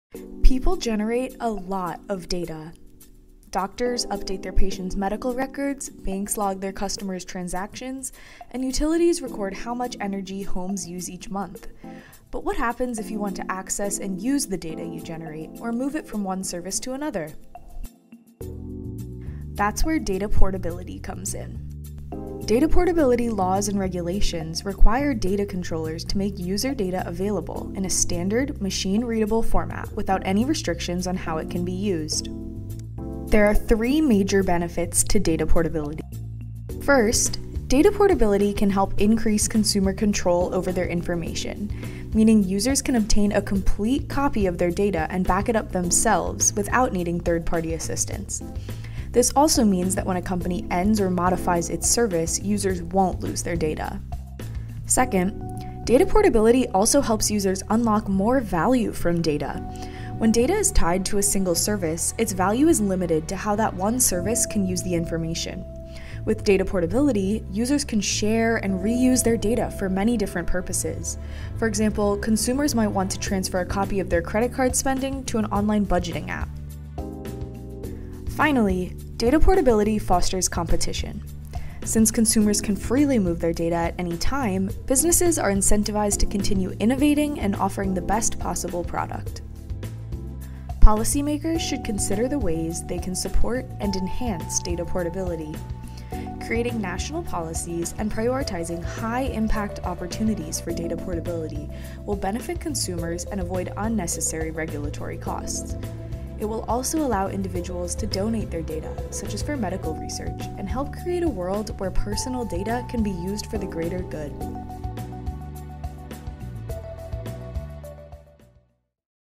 How Can Data Portability Benefit Consumers?
Data portability laws and regulations require data controllers to make user data available in a standard, machine-readable format without any restrictions on how it can be used.

Policymakers should consider the ways they can support and enhance data portability.